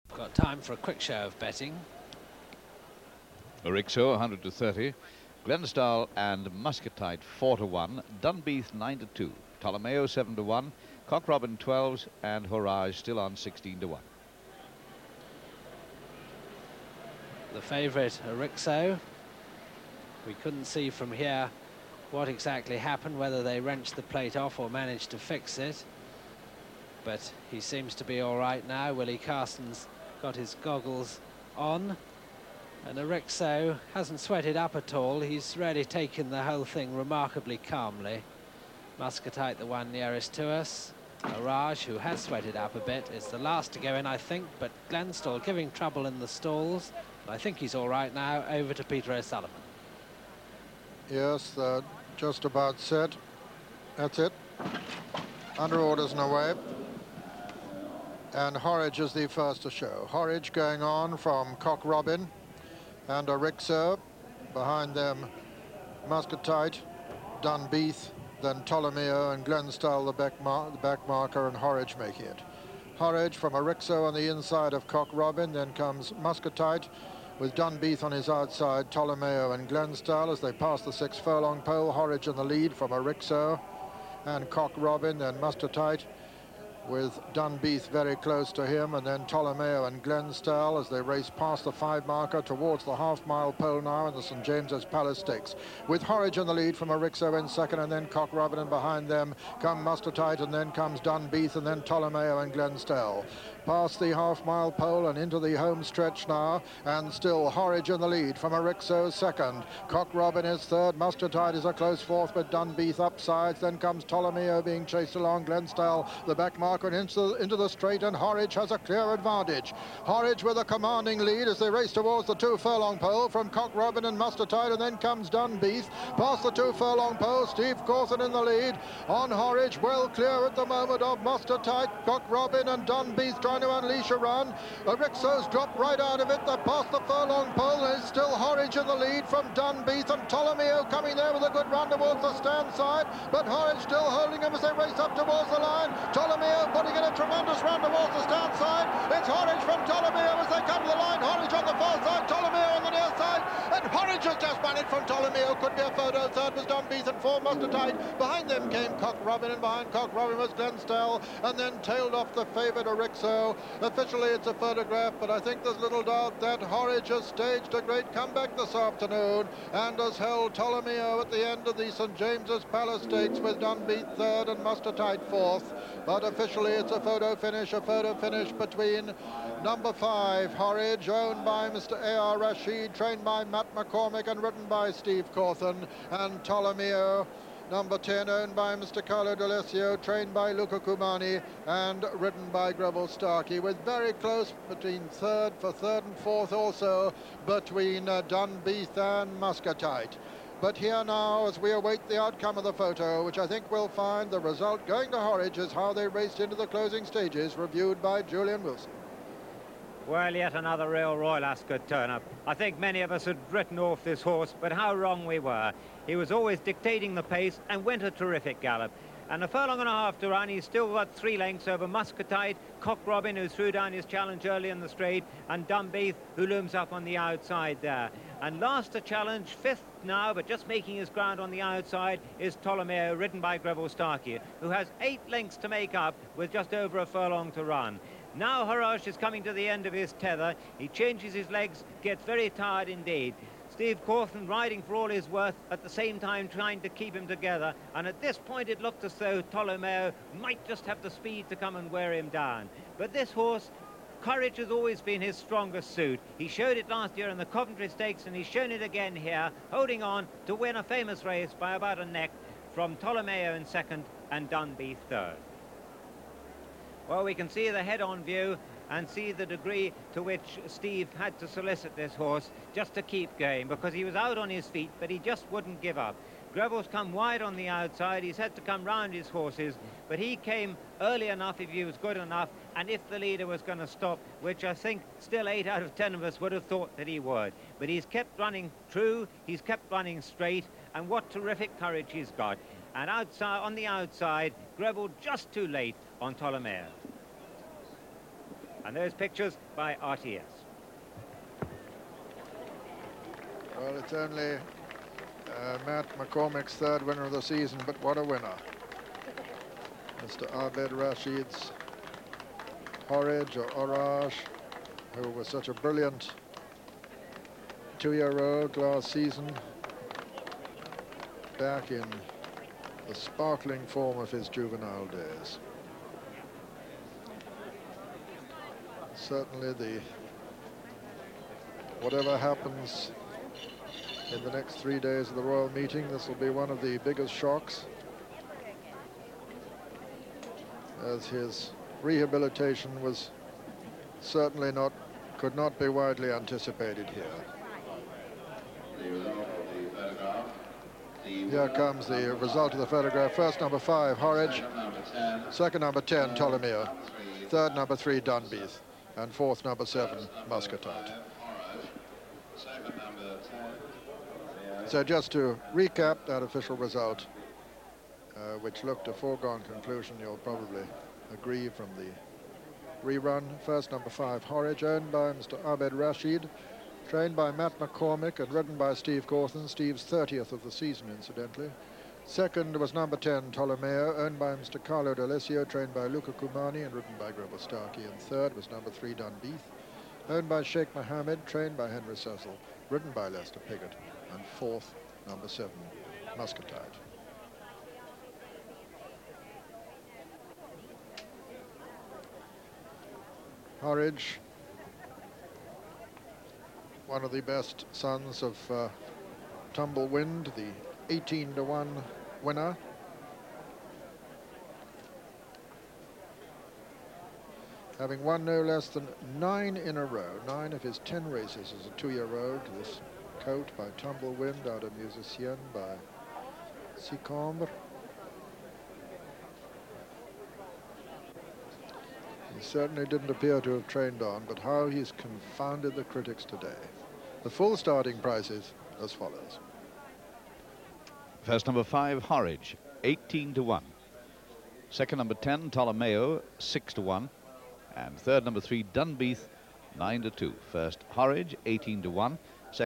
1983 St James's Palace Stakes Horage Beats Tolomeo Inc Post Race
1st Horage 2nd Tolomeo 3rd Dunbeath
3.45 14th June 1983  Distance 1m  Going Good/Firm Course Ascot